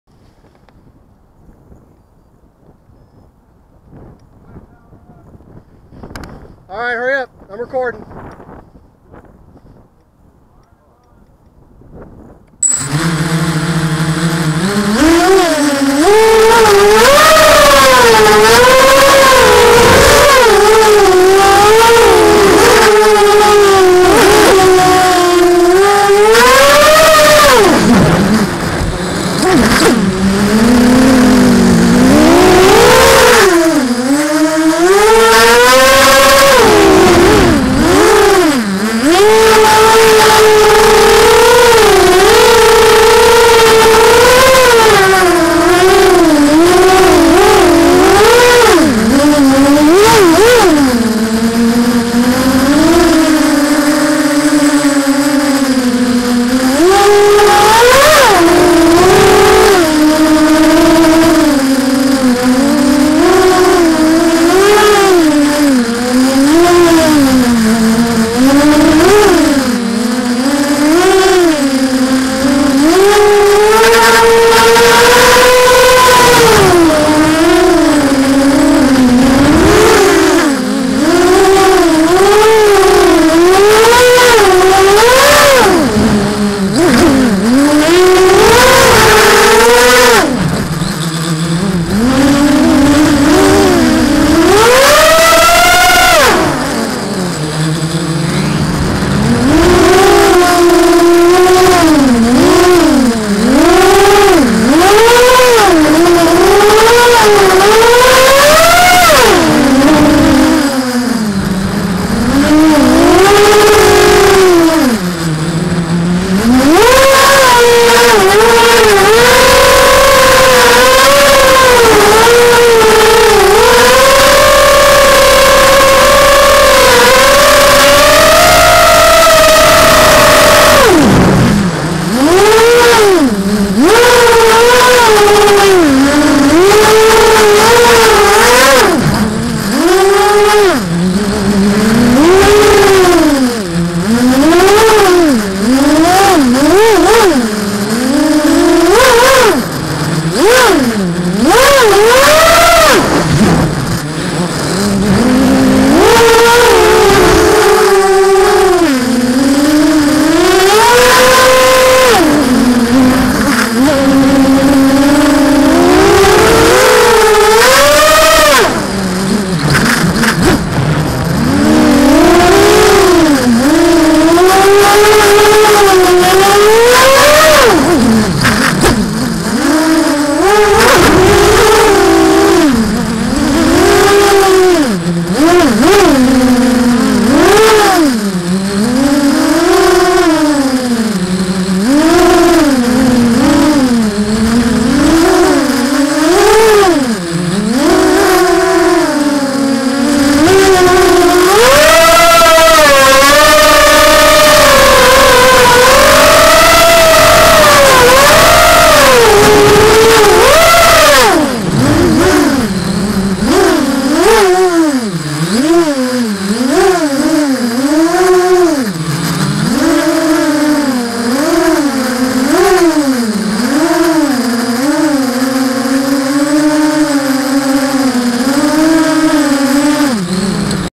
BetaFlight Acro+ 4S EMAX RS2205 2300kv
Sorry about the jello in the video - tried a new Mobius mount with anti-vibration gel and it seems the gel should be renamed to Vibration Gel...

Frame: Strictly Racing Drones SR6
Motors: EMAX RS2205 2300v "Red Bottoms"
Props: DAL 6045 (V1s sorry about all the vibrations in the video!!)
ESC: Blizzard 20A (Blheli 14.3 damped light enabled)
FC: X-Racer F303 (Betaflight 2.5.3)
Battery: Bonka 4S 1300mah 75C

BetaFlight 2.5.3
Lux Float - Stock PIDs
P/R 0.65 (expo 70) Yaw .55 (expo 20)
Acro+ (offset=40, factor=60)